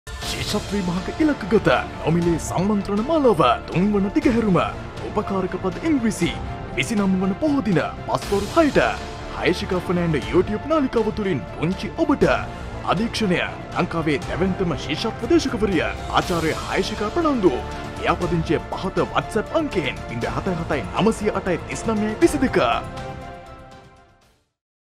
"මහා ඉංග්‍රීසි සම්මන්ත්‍රණය"සැප්තැම්බර් 29Hayeshika fernando youtube නාලිකාව ඔස්සේ සජීවීව පැවැත්වේ.
2023 මෙවර ශිෂ්‍යත්ව විභාගයට පෙනී සිටින
මුළු දිවයිනේ ම දරුවන් වෙනුවෙන්✍️

🥇"සම්පූර්ණයෙන් නොමිලේ"🥇
පැවැත්වෙන උපකාරක පද

🔸"මහා ඉංග්‍රීසි සම්මන්ත්‍රණය"🔸

▶️සැප්තැම්බර් 29 ප.ව.6.00ට
Hayeshika fernando youtube නාලිකාව ඔස්සේ සජීවීව පැවැත්වේ.

🏆අධීක්ෂණය ලංකාවේ අංක 01 ශිෂ්‍යත්ව දේශකවරිය ආචාර්ය හයේශිකා ප්‍රනාන්දු

💫සම්බන්ධවීමට දරුවාගේ නම/ශ්‍රේණිය/දිස්ත්‍රික්කය සදහන් කර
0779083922 අංකයට Whatsapp ඔස්සේ යොමුකරන්න.

🏅whatsapp සමූහයන් හා එකතුවීමට පහත ලින්ක් භාවිතා කරන්න.